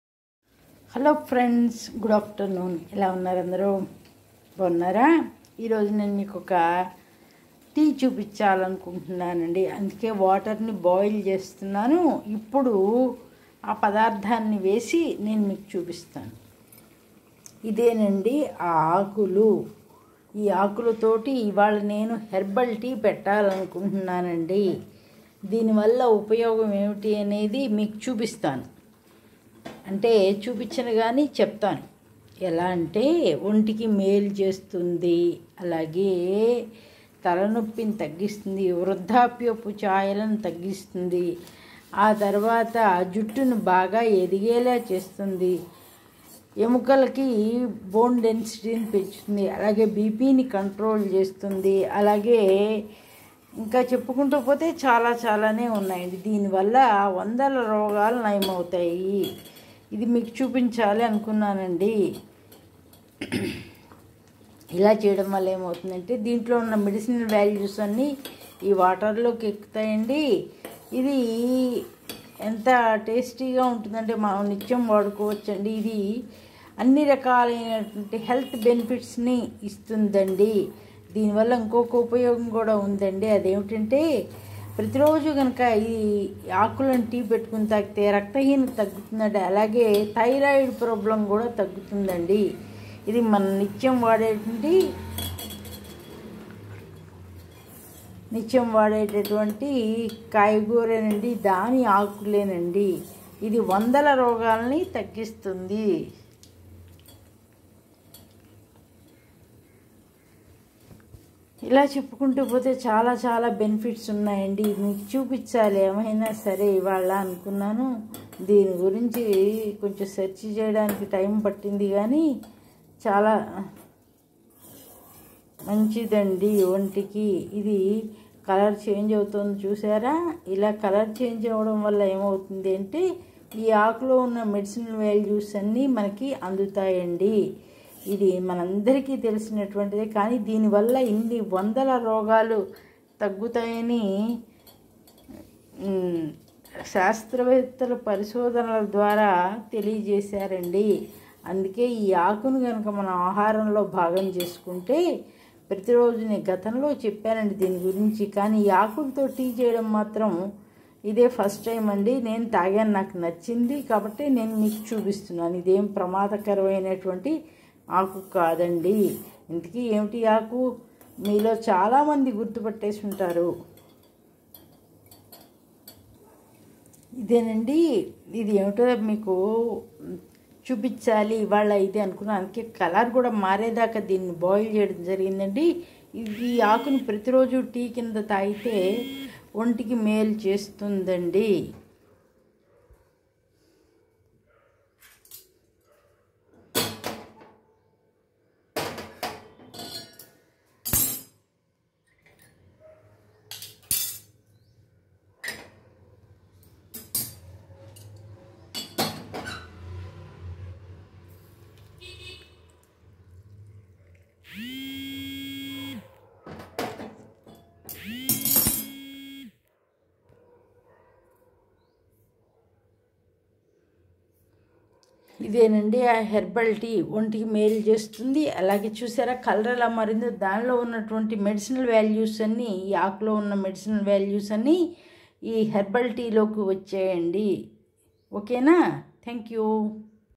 హెర్బల్ టీ విత్.......
హాయ్ ఫ్రెండ్స్ ఎలా ఉన్నారు? ఈరోజు నేను మీకు ఒక హెర్బల్టి చూపించాలి అని అనుకున్నాను అండి అందుకే కొంచెం టైం తీసుకోవడం జరిగింది కొద్దిగా లేట్ అవ్వడానికి కారణం కూడా అదే ఇంతకీ ఏమిటంటే ఈ హెర్బల్టి వల్ల ఎన్నో రకాలైనటువంటి ఆరోగ్య ప్రయోజనాలతో పాటు ఆకులో ఎన్నో విటమిన్స్ మినరల్స్ శరీరానికి కావలసిన పోషకాలు మాక్సిమం అందుతాయి అండి. ఈ హెర్బల్టి వల్ల మనందరికీ తెలిసిందే నండి ప్రకృతిలో దొరికేటటువంటి ఆక్సిడెంట్ అండి ఈ హెర్బల్ వల్ల మనకి అనేక ప్రయోజనాలు ఏమి ప్రయోజనం అంటే ఒకటి మొట్టమొదటి కంటిచూపులు మెరుగుపరుస్తుందండి కంప్యూటర్స్   లేదంటే లాప్టాప్ ఇలాంటి వాటిని ముందు వర్క్ చేసుకునే వాళ్ళకి బాగా ఉపయోగపడుతుందండి. ఎందుకంటే ఐకి ఎక్కువ శ్రమ ఇచ్చేది గాడ్గేట్స్ ఉపయోగించడం వల్లే అందుకని అలాగే చర్మం ముడతలు రానీయదండి అలాగే జుట్టును బాగా పెరిగేలా చేస్తుంది చర్మకాంతిని వృద్ధి చేస్తుంది ఎముకలకు బోన్ డెన్సిటీని పెంచుతుంది రక్తహీనతను తగ్గిస్తుంది ఆ తర్వాత ఆ మెయిన్ ఇంకోటి ఏమిటంటే థైరాయిడ్ ప్రాబ్లం కూడా కంట్రోల్ చేస్తుందండి ఈ హెర్బల్ టీ తాగడం వల్ల ఒంటికి చాలా మేలు చేస్తుంది ఇంతకీ ఏమిటి అనుకుంటున్నారు మరి ఏదో కాదండి వందల రోగాలు నయం చేస్తే ములగాకండి దానితో నేను టీ పెట్టి చూపించాను మీకు రెగ్యులర్గా తాగితే కంటి సమస్యలు ఒంటి సమస్యలు అన్ని సమస్యలకి సమాధానం చెబుతుంది.